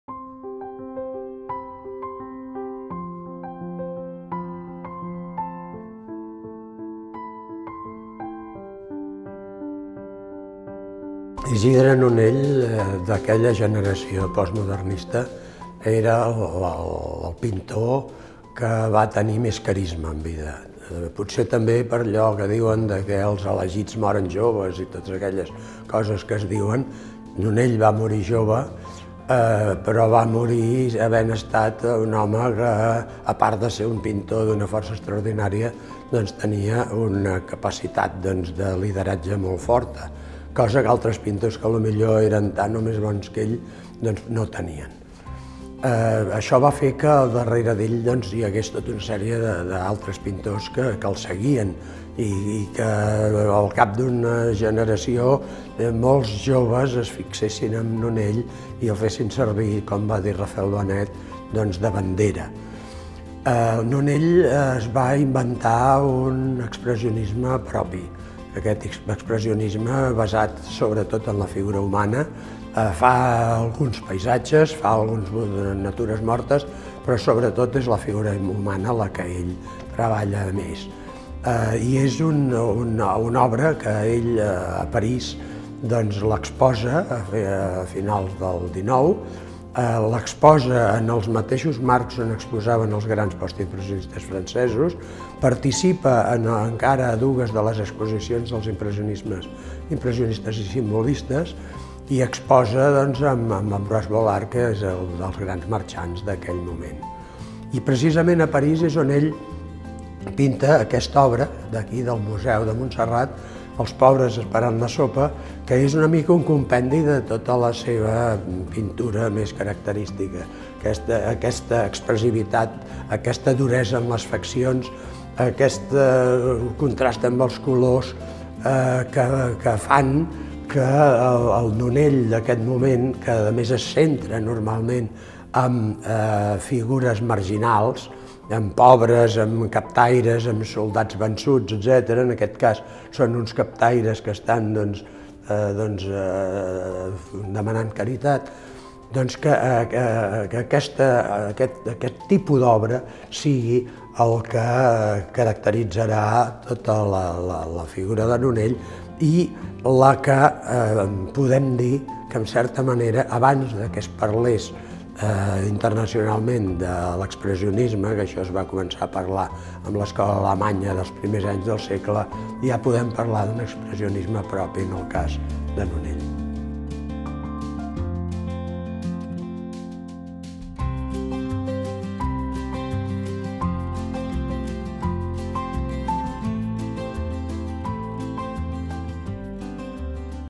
La pintura d'Isidre Nonell (7/12 Pintura catalana. El Modernisme)
L'historiador de l'art Francesc Fontbona, director de l'obra 'Pintura catalana. El Modernisme', d'Enciclopèdia Catalana, parla d'Isidre Nonell.